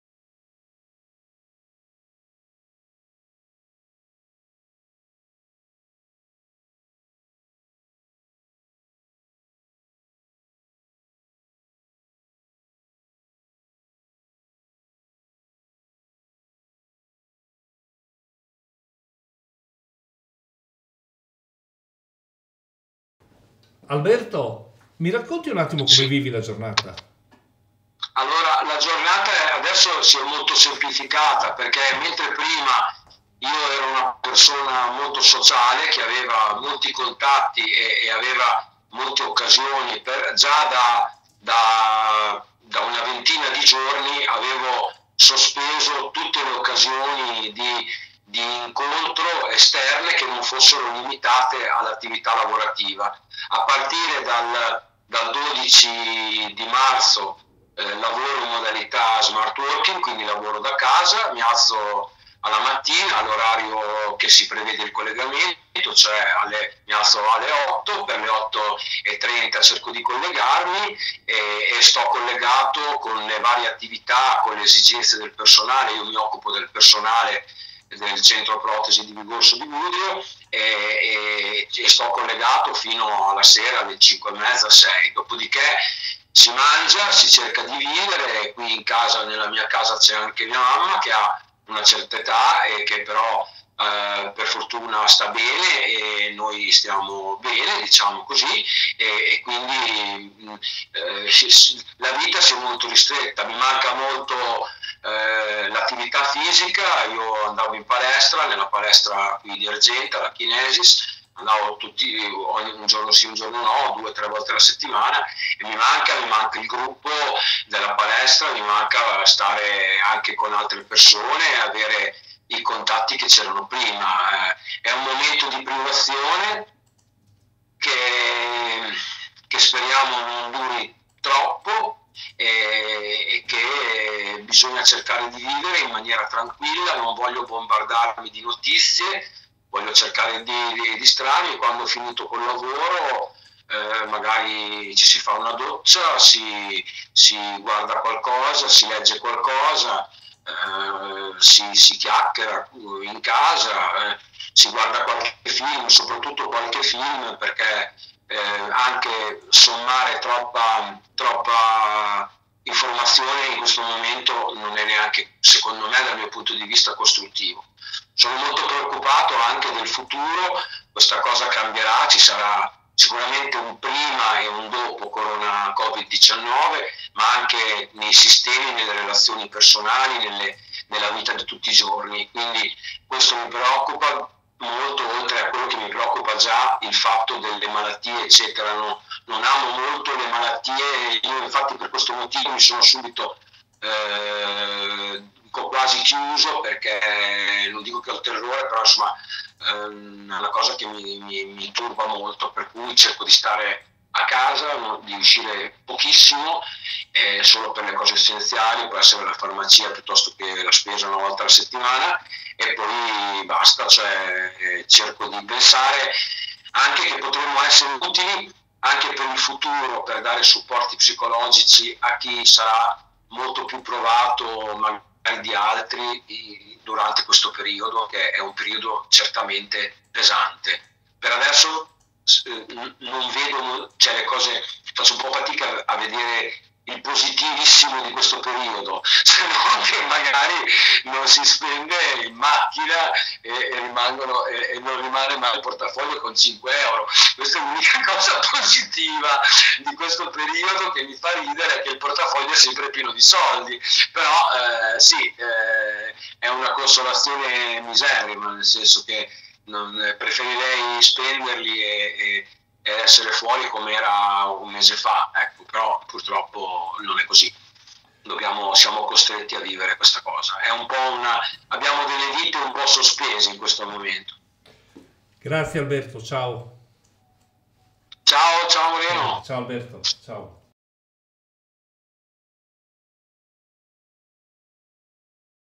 Come Vivi la tua Giornata
Alberto Vistoli segretario del Lions Club di Argenta e Terre del Primaro. Ci racconta come passa la sua giornata in questo periodo di quarantena.